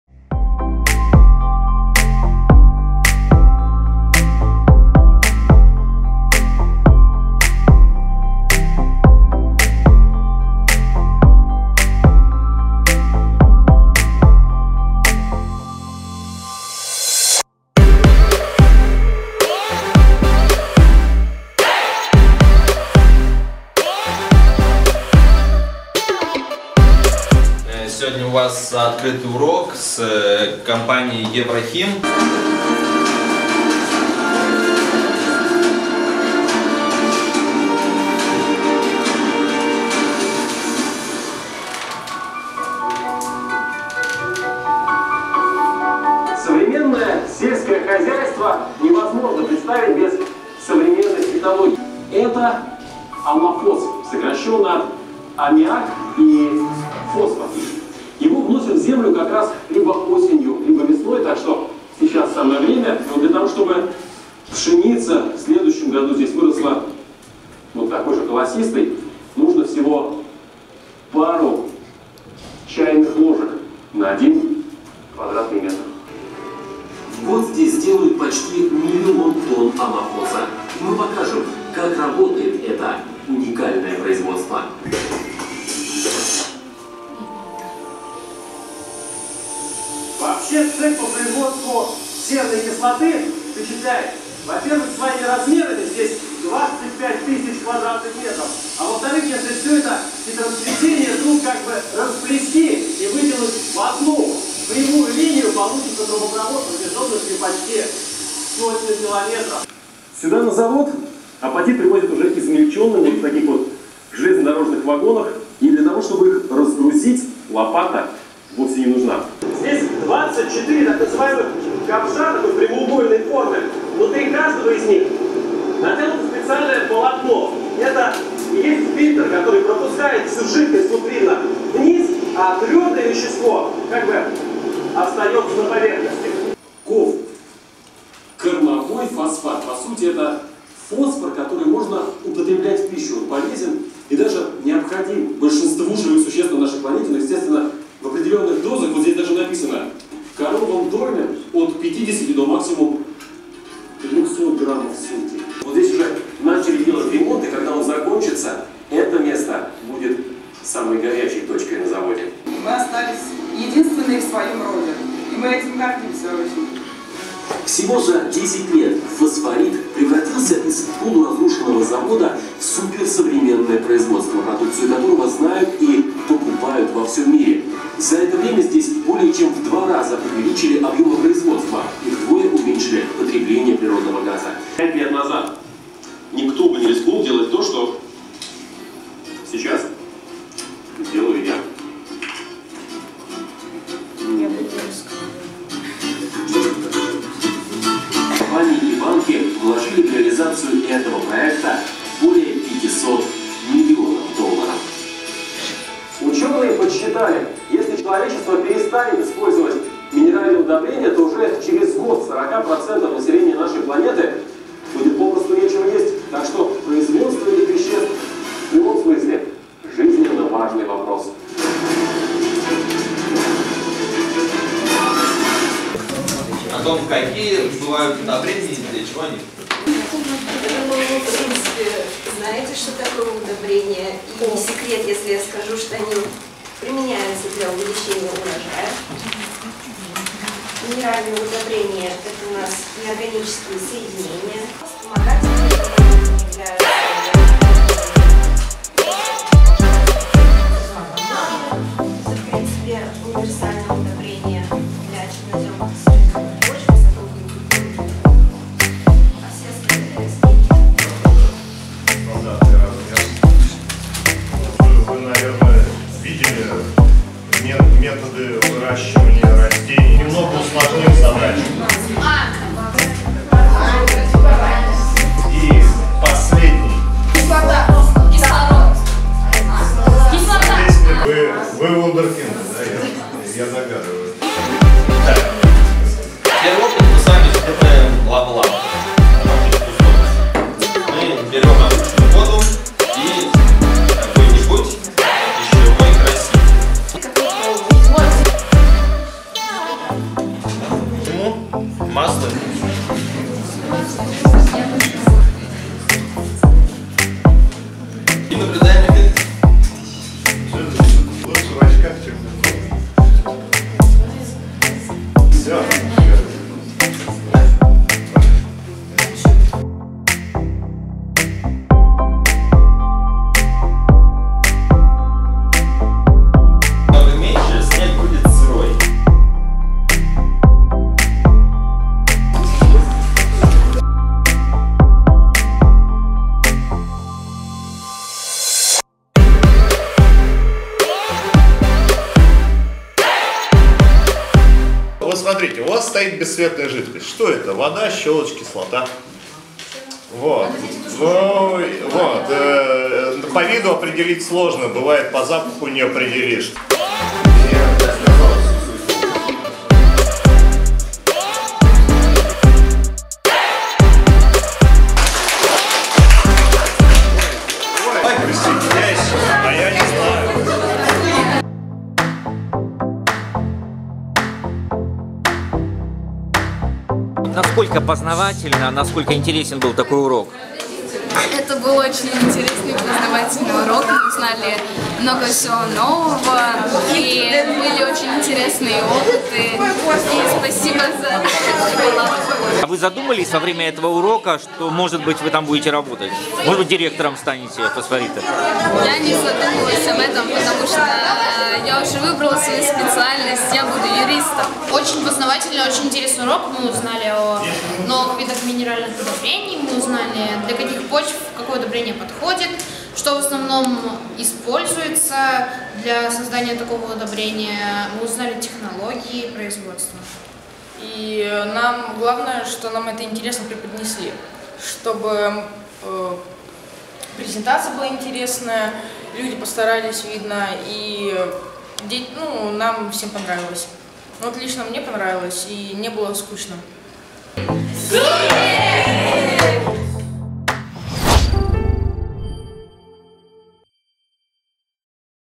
Кингисеппские школьники подробно разобрались в производстве «Фосфорит»
Подробнее: городкингисепп.рф